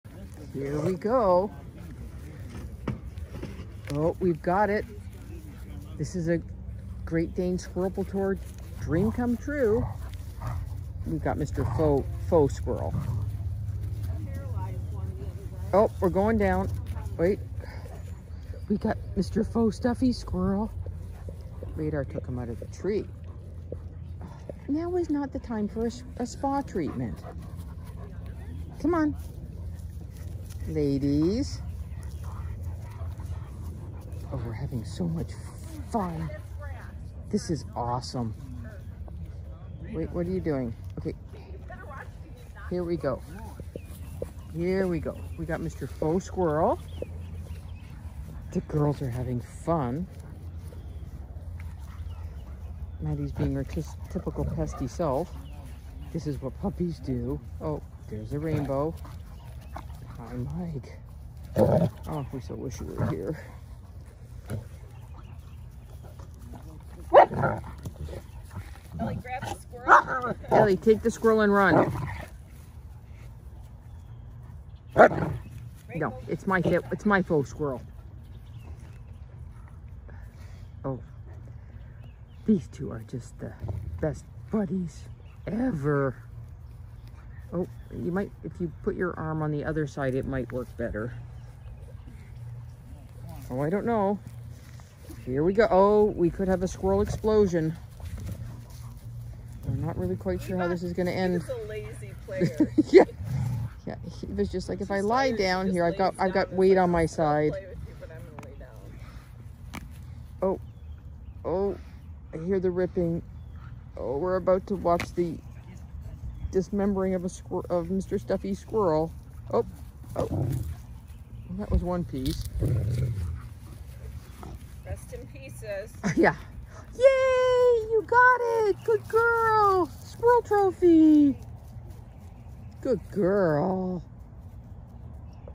Playful Great Danes Have Fun With Squirrel Toy Tug Of War
Eva and Maddie love playing together and what more fun than playing with Mr Squirrel?  Watch and laugh as Ellie watches as Mischief and Mayhem have great fun tugging on Mr Squirrel Toy, who is now resting in pieces.